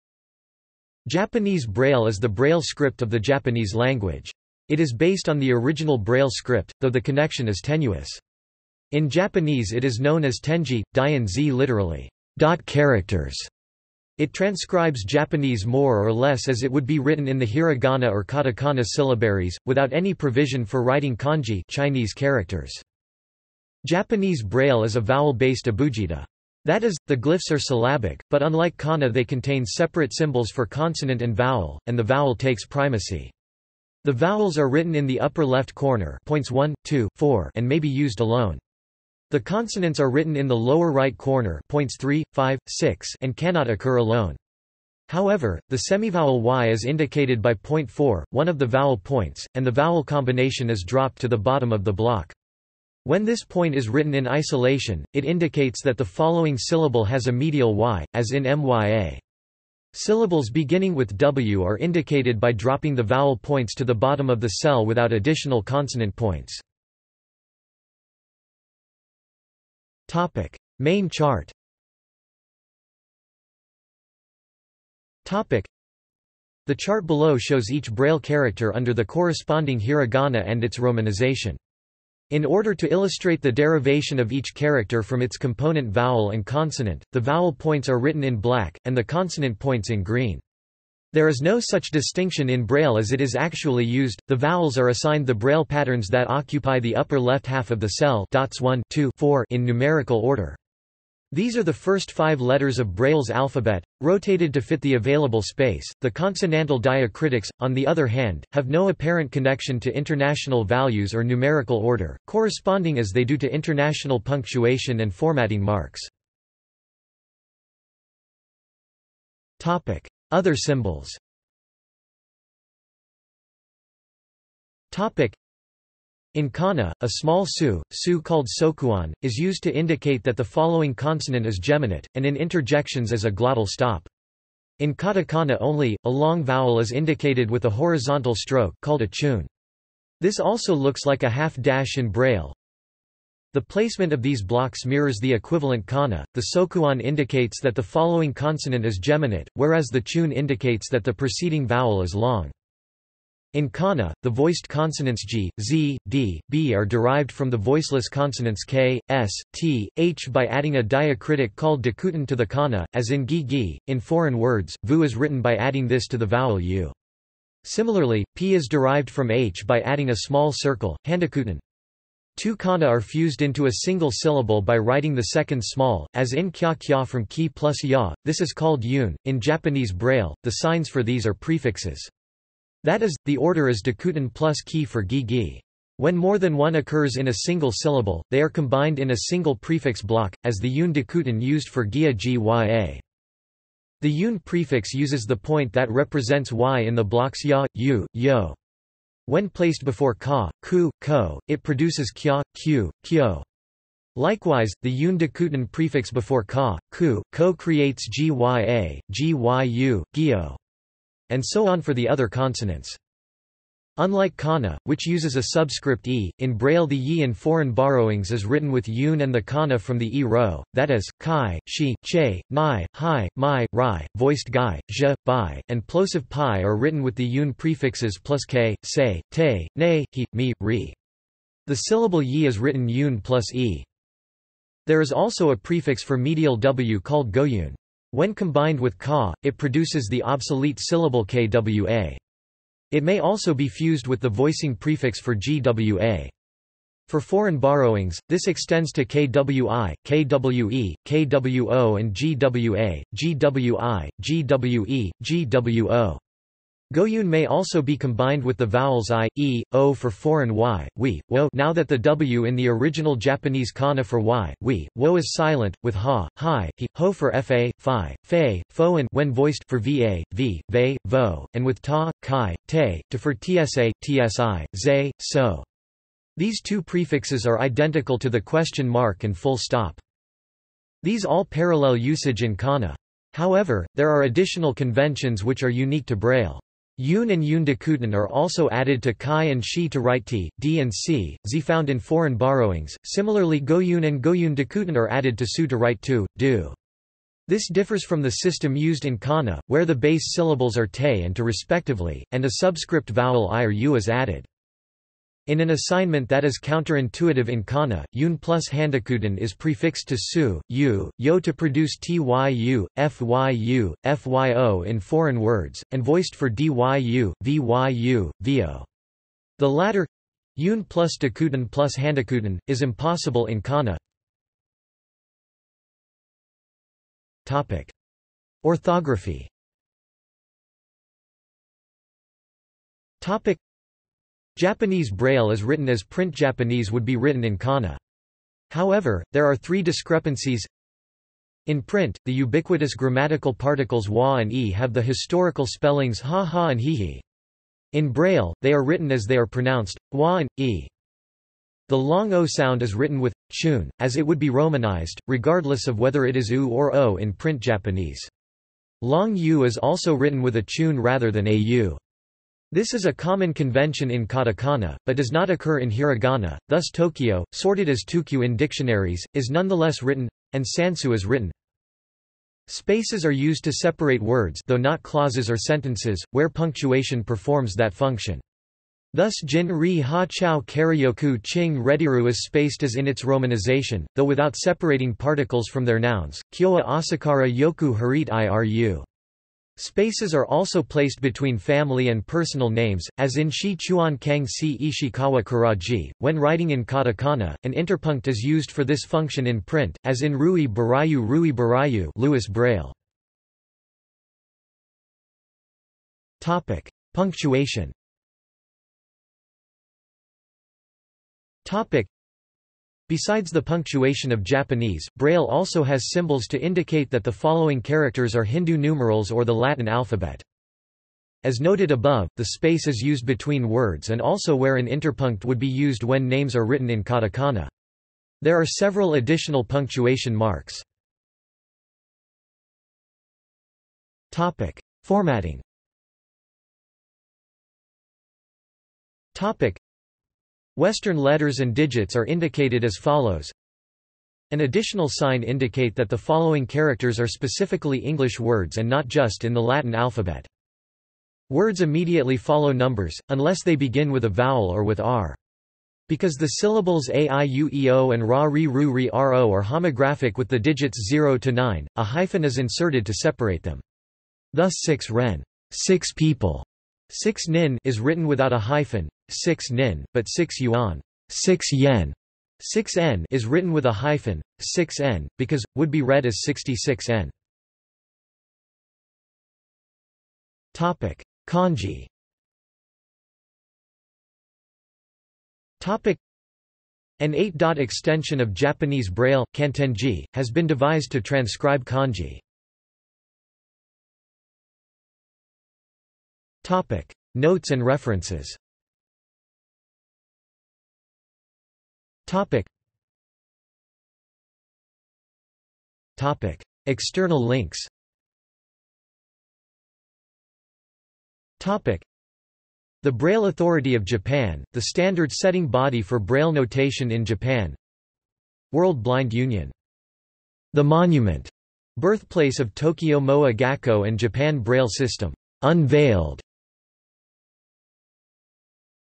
Japanese Braille | Wikipedia audio article
This is an audio version of the Wikipedia Article:
Japanese Braille

SUMMARY
Japanese Braille is the braille script of the Japanese language. It is based on the original braille script, though the connection is tenuous. In Japanese it is known as tenji (点字), literally "dot characters".  It transcribes Japanese more or less as it would be written in the hiragana or katakana syllabaries, without any provision for writing kanji (Chinese characters).
Japanese Braille is a vowel-based abugida. That is, the glyphs are syllabic, but unlike kana they contain separate symbols for consonant and vowel, and the vowel takes primacy. The vowels are written in the upper left corner (points 1, 2, 4) and may be used alone. The consonants are written in the lower right corner (points 3, 5, 6) and cannot occur alone. However, the semivowel y is indicated by point 4, one of the vowel points, and the vowel combination is dropped to the bottom of the block. When this point is written in isolation, it indicates that the following syllable has a medial y, as in mya. Syllables beginning with w are indicated by dropping the vowel points to the bottom of the cell without additional consonant points.